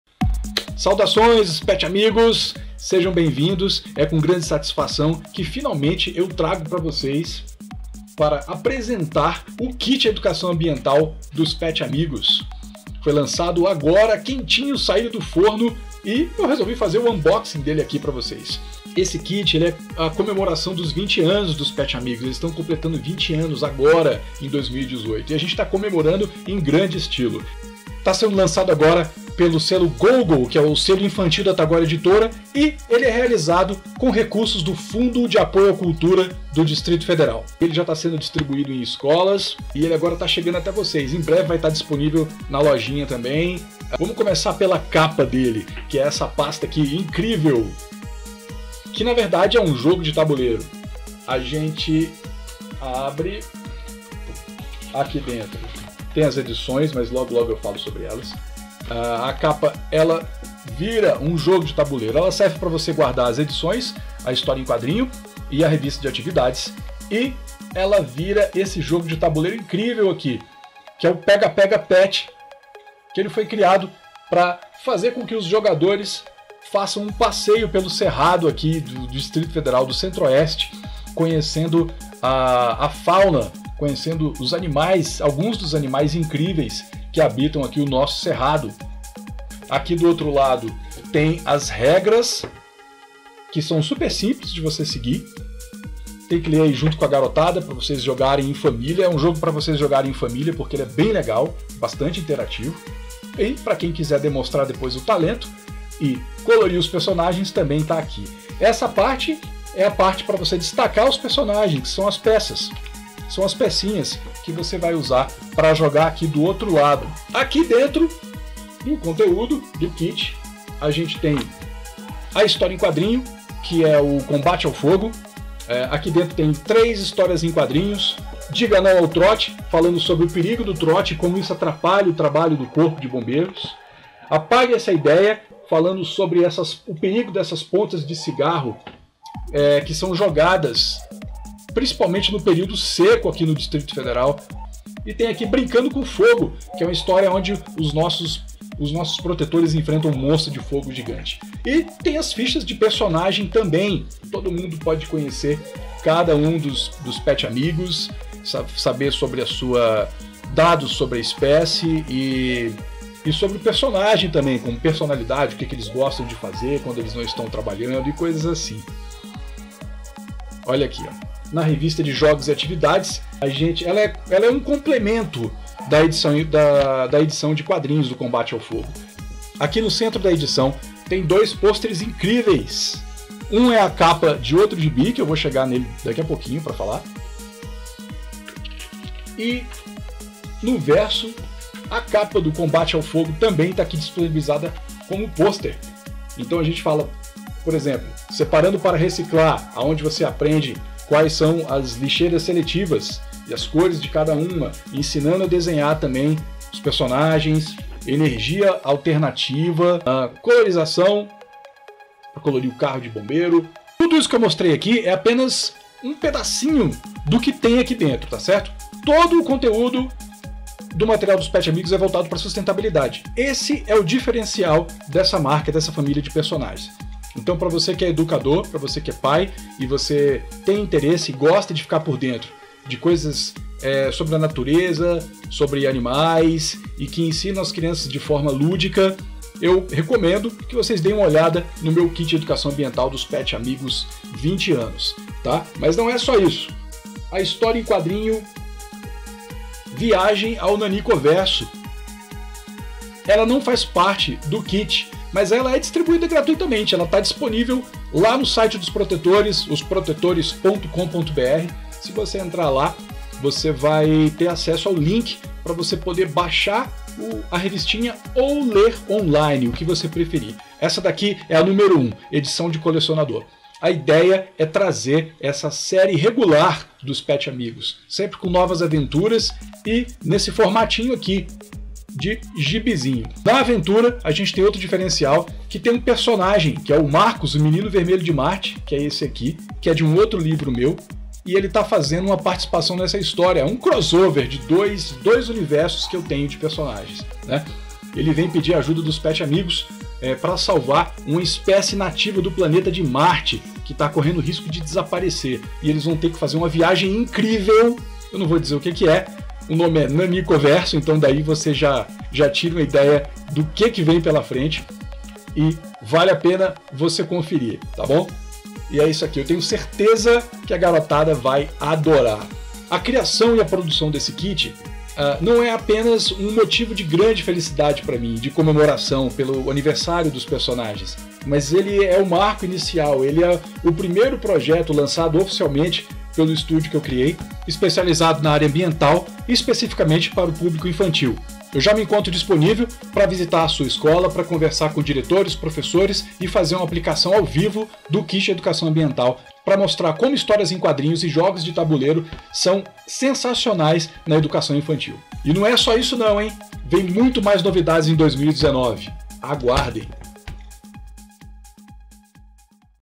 Unboxing: Kit PET Amigos de educação ambiental!
Aprender brincando é sempre mais divertido! O livro "Jogos e Atividades" dos PET Amigos, contém 32 jogos e atividades para quem sabe tudo sobre a natureza e o meio ambiente!

Ele faz parte do Kit de Educação Ambiental que será lançado em comemoração aos 20 anos dos Protetores da Existência na Terra, criados pelo quadrinista Nestablo Ramos, sob o selo Gôgo da Tagore Editora.

Projeto realizado em parceria com Sinergia Cultural